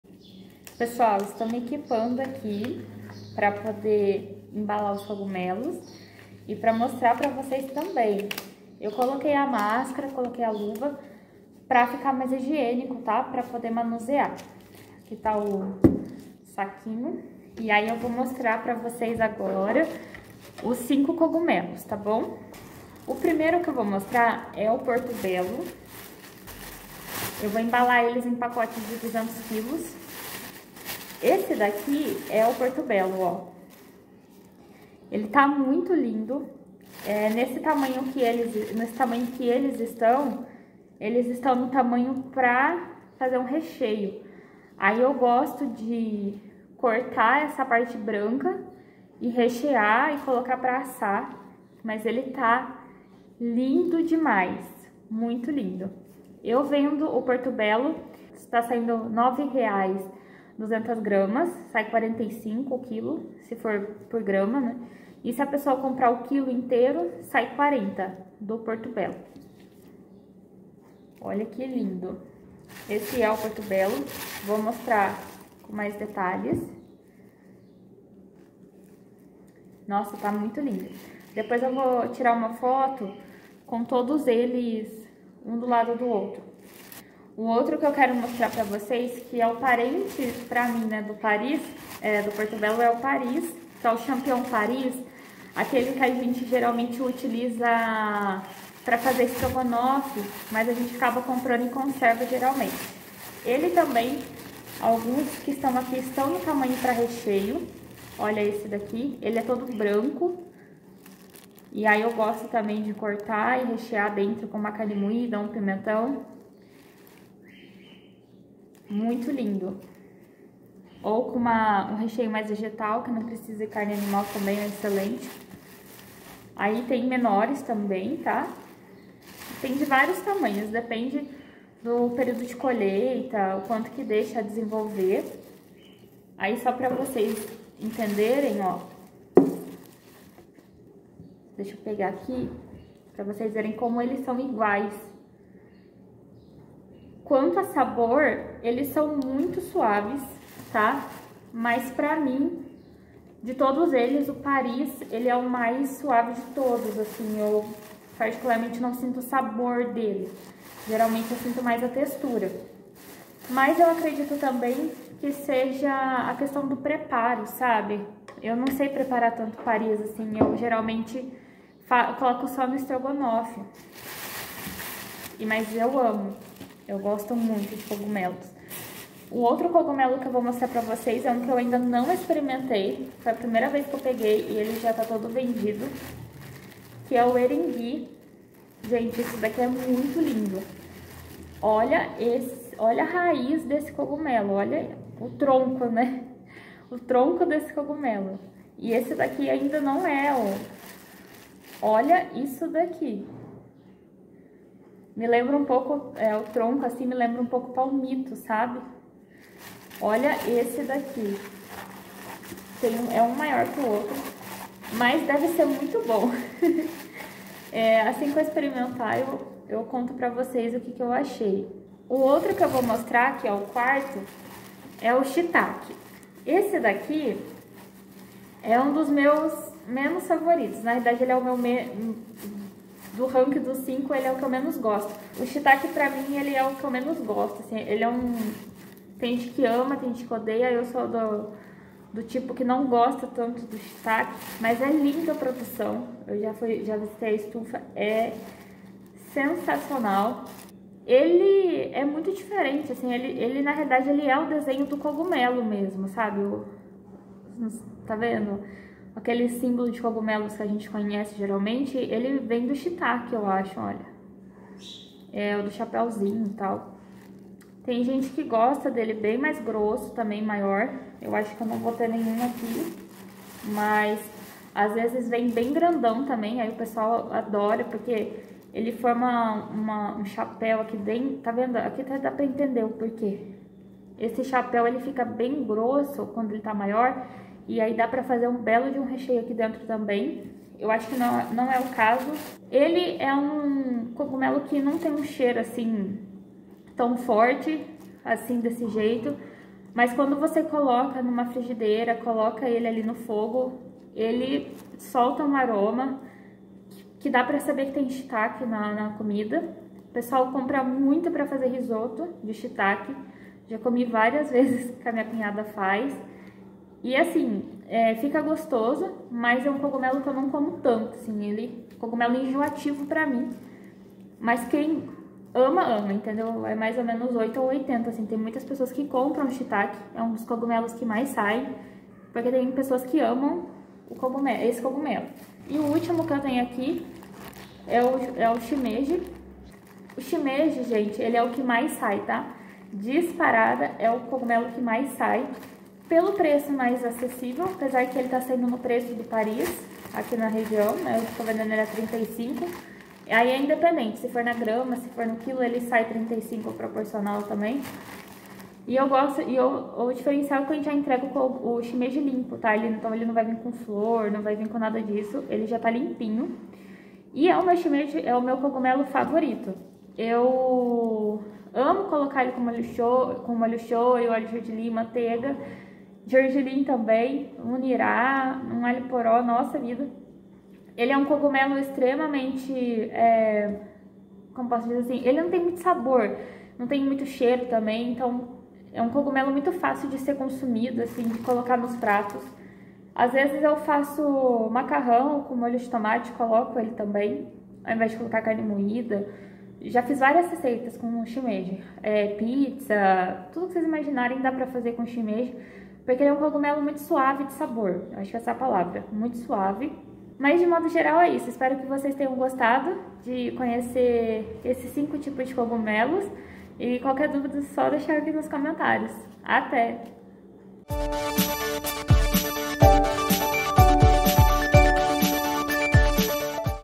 5 TIPOS DE COGUMELOS que vendo na Empório
Oi pessoal, tudo bem? Hoje vim mostrar 5 tipos de cogumelos comestíveis para vocês :)

Os cogumelos que mostro no vídeo são:
1. Champignon Paris
2. Portobello
3. Shitake
4. Shimeji Branco
5. Eryngui